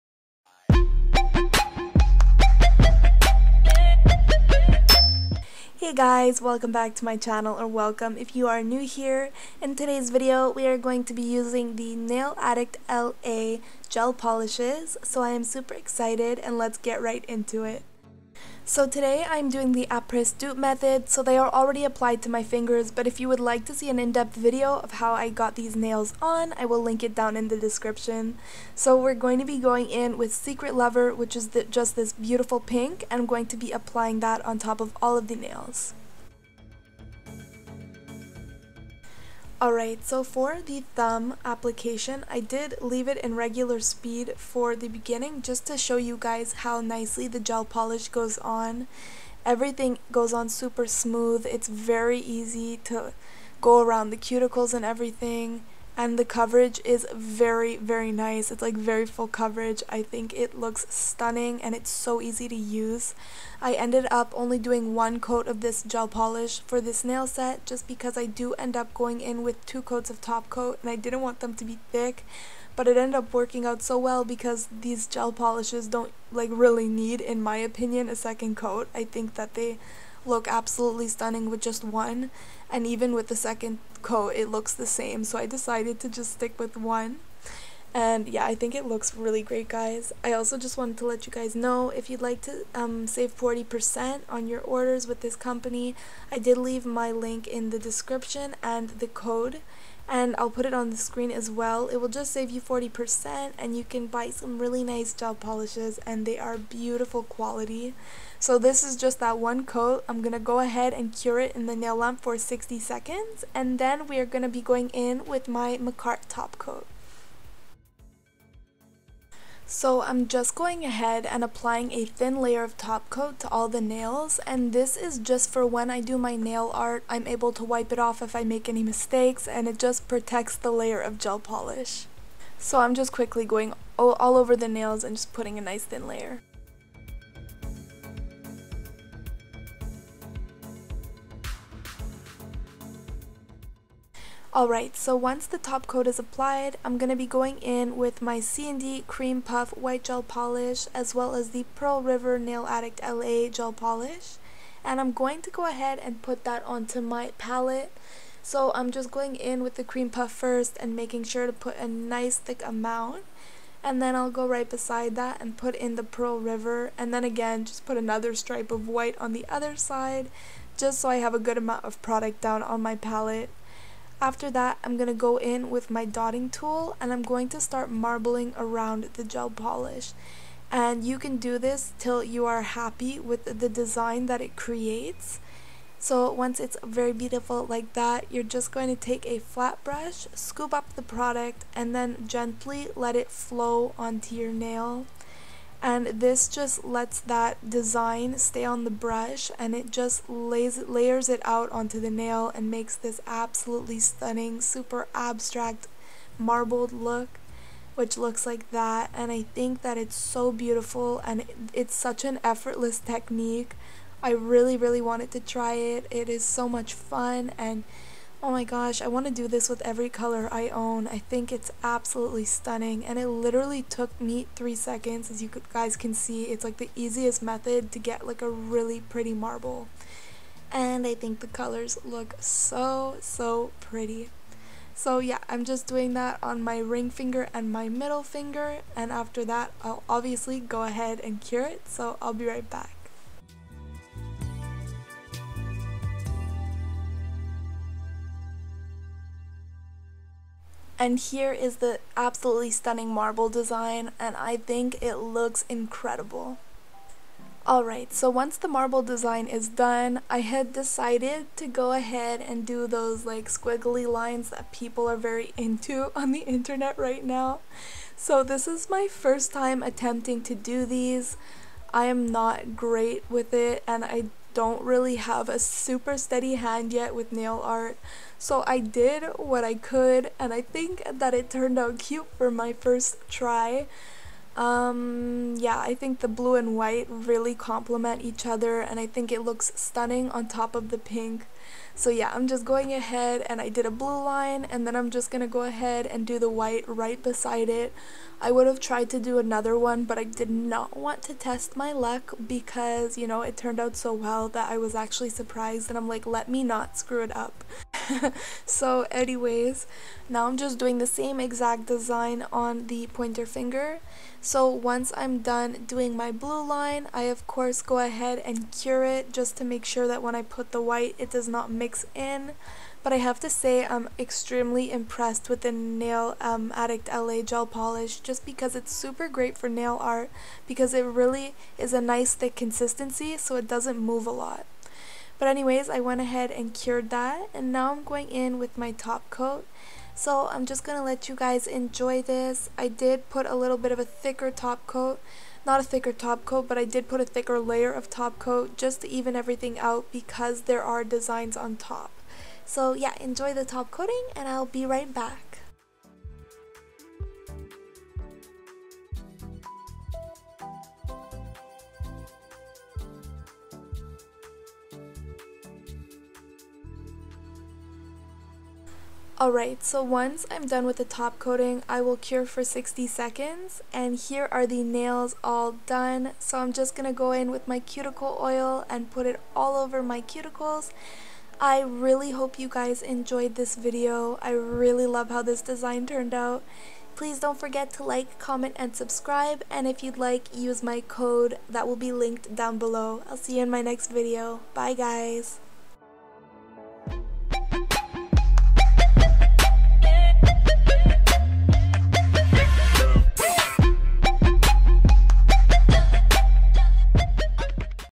HOW TO MARBLE GEL POLISH | Nail Addict LA gel polish tutorial!
This gel polish is litterally one of the best ones I've used, it goes on beautifully and gives amazing coverage with even just one coat! Im obsessed ! I hope you find this video helpful and if you guys are interested, I will leave my code down below and you guys can save some money! Love you all sooo much xoxoxoxo

products used:
makartt full cover tips